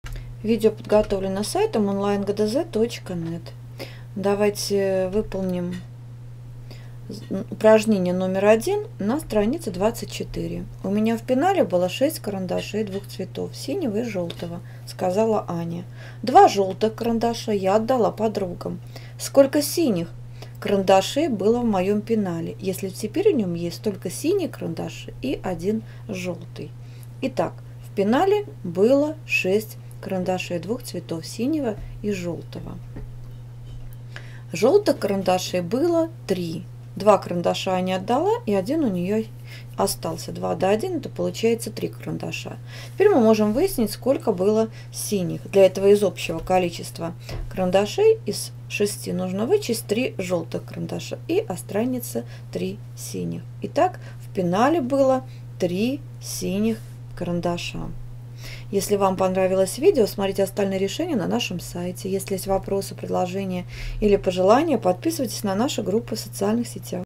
Страница 24 Задание 1 – Математика 2 класс (Моро) Часть 1
В упражнении №1 на странице 24 необходимо уметь решать логические задачи на нахождение неизвестного слагаемого. Посмотрите наше видео, чтобы всё стало понятно.